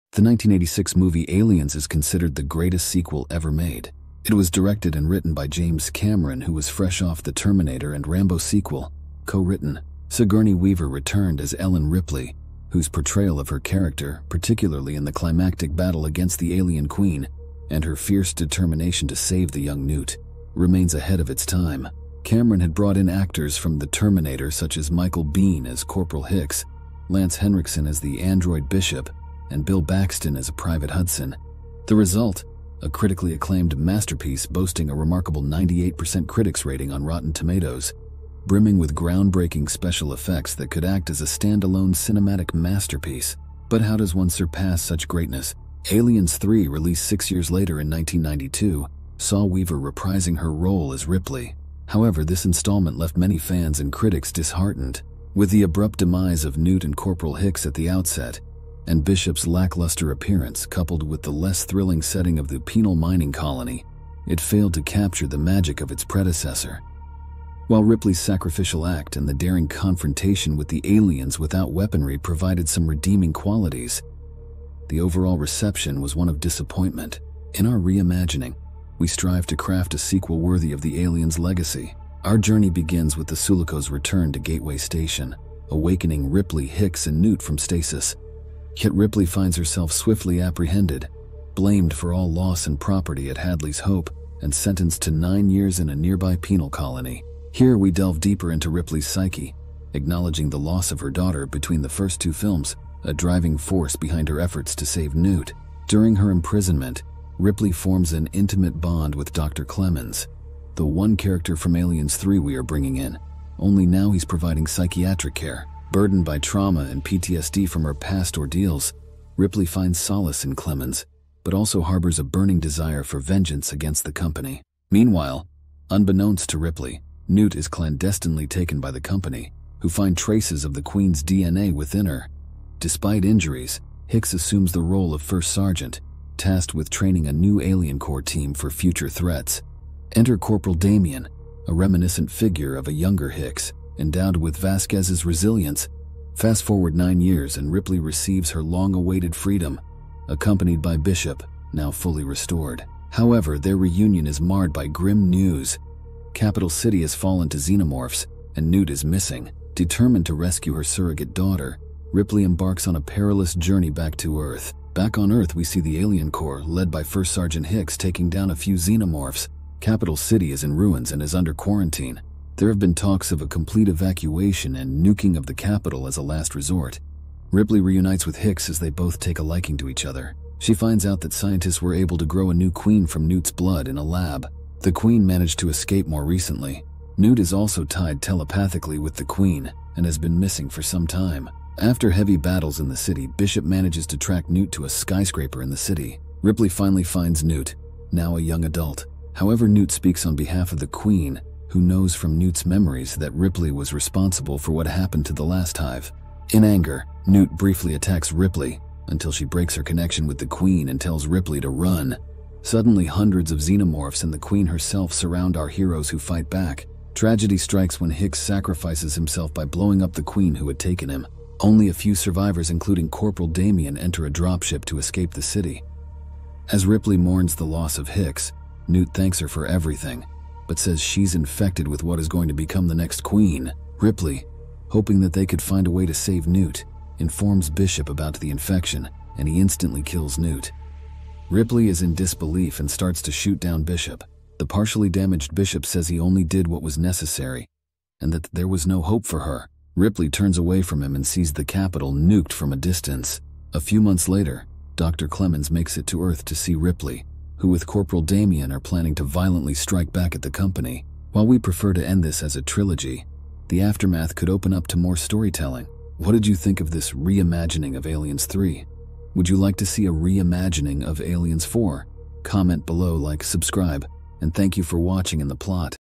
Reimagining Alien 3 (1992) with Ellen Ripley, Corporal Hicks, Bishop, and Newt
Alien 3, released six years after the critically acclaimed Aliens (1986), saw Sigourney Weaver (Ghostbusters) reprising her role as Ripley and introducing Charles Dance (Game of Thrones) as Dr. Clemens. However, this installment left many fans and critics disheartened. With the abrupt demise of Newt and Corporal Hicks at the outset and Bishop's (Lance Henriksen) lackluster appearance, coupled with the less thrilling setting of the Penal Mining Colony, it failed to capture the magic of its predecessor.

While Ripley's sacrificial act and the daring confrontation with the aliens without weaponry provided some redeeming qualities, the overall reception was one of disappointment. In our reimagining, we strive to craft a sequel worthy of the Aliens legacy. Our version stars:

Sigourney Weaver as Ellen Ripley
Winona Ryder as Newt (child played by Carrie Henn)
Michael Biehn as First Sgt. Hicks
Lance Henriksen as Bishop
Charles Dance as Dr. Clemens

Some images are from Edward Scissorhands (1990) and Hard Target (1993).